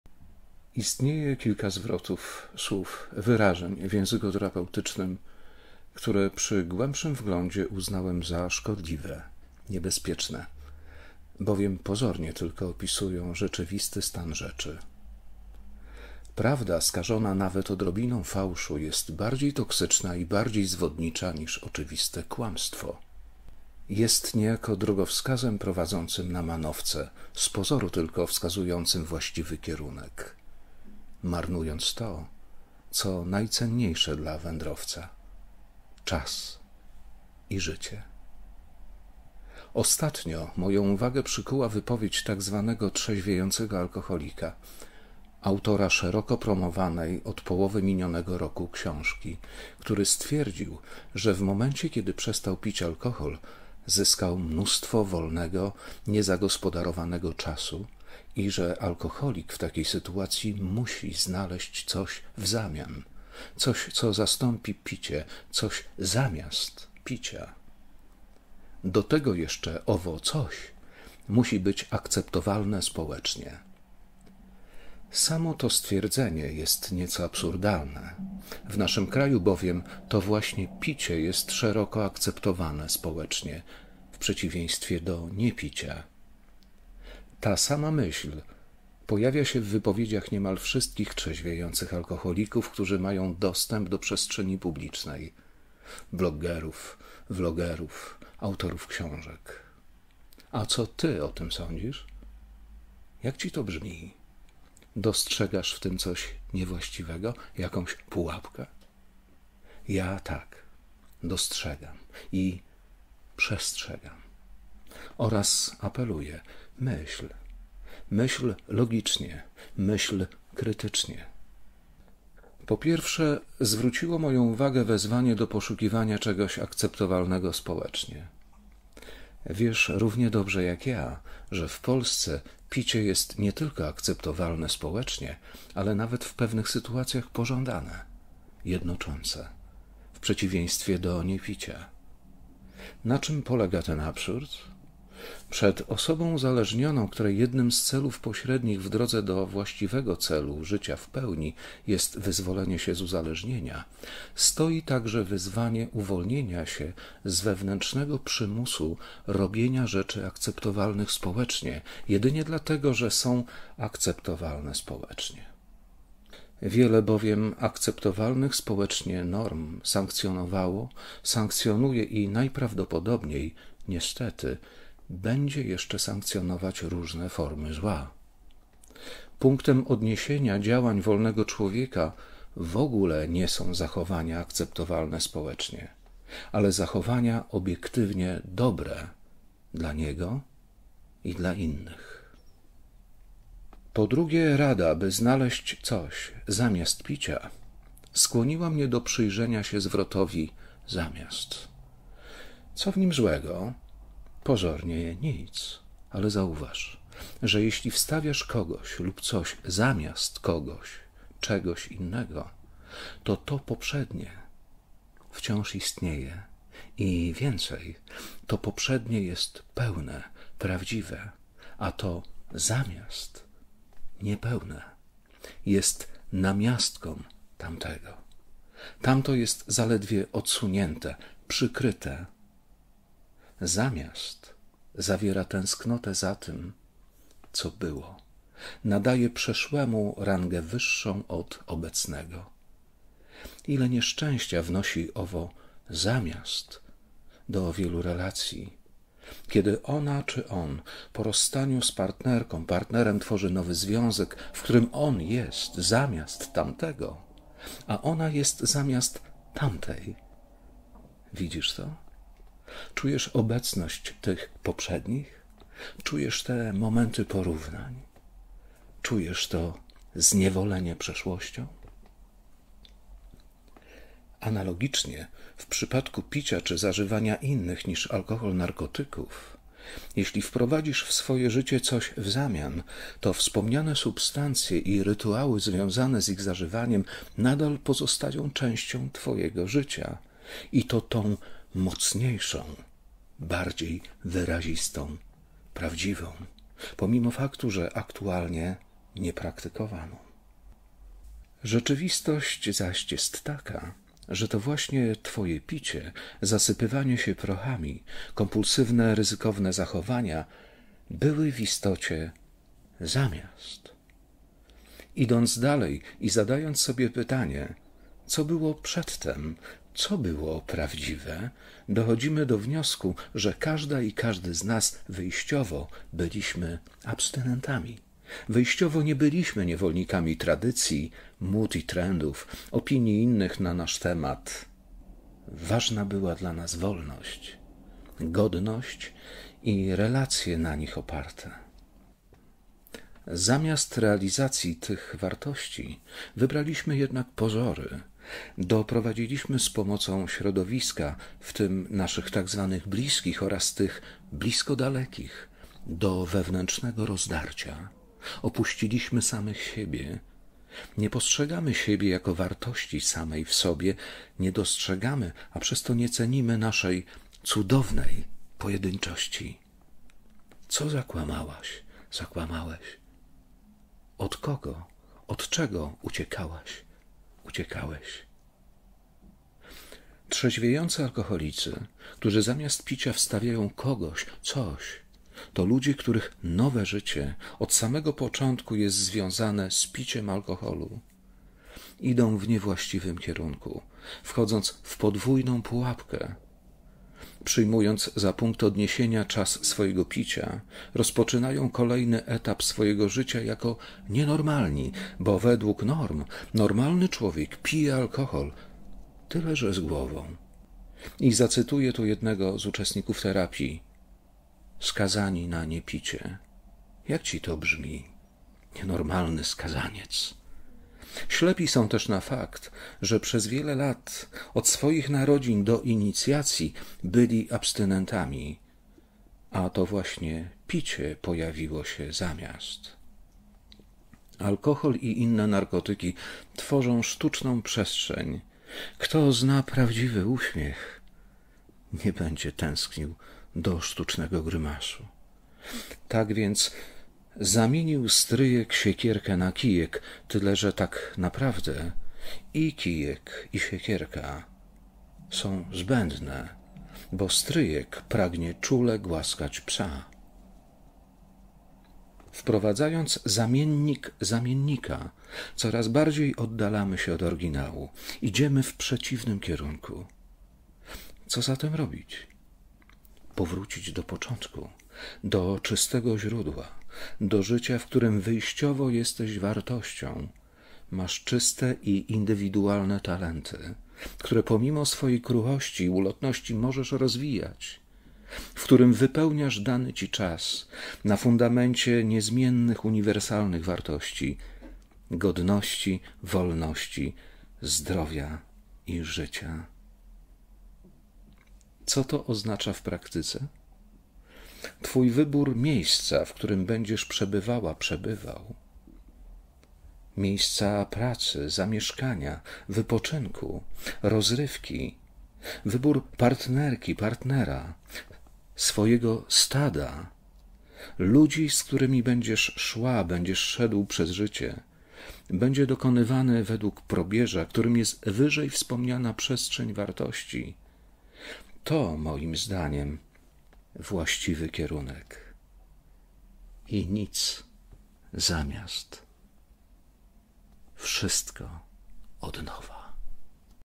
Niebezpieczne zwroty? "ZAMIAST" [#13] Jarosław Wajk - TERAPIA UZALEŻNIEŃ
Jednym z niebezpiecznych, prowadzących na manowce zwrotów jest słowo "zamiast". Równie niebezpieczne jest zachęcanie do znalezienia aktywności "akceptowalnych społecznie". Wyjaśnię Ci, dlaczego tak sądzę.
Więcej: jarosławwajk.pl